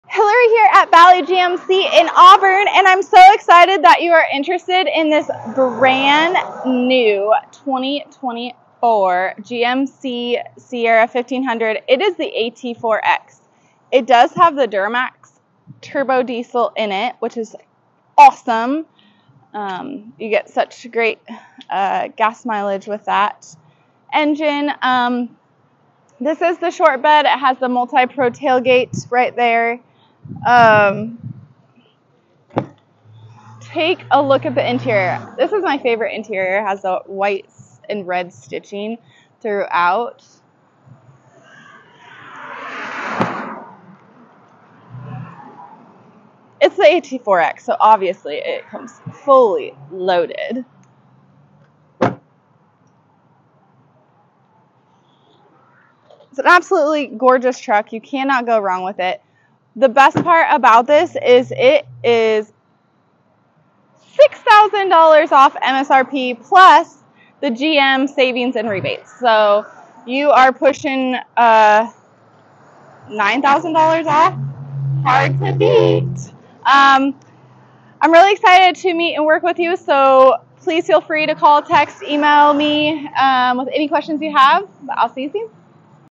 Check Out the New 2024 GMC Sierra 1500 AT4X - Your Ultimate Off-Road Adventure Awaits!
Ready for your next adventure? The New 2024 GMC Sierra 1500 AT4X is here, and it’s built to take you anywhere you want to go! Join us at Valley GMC in Auburn as we show you all the cool features of this off-road powerhouse. From its rugged design to its comfortable, tech-savvy interior, this truck has everything you need for work and play. Come by Valley GMC and see why the Sierra 1500 AT4X is the perfect companion for your outdoor lifestyle!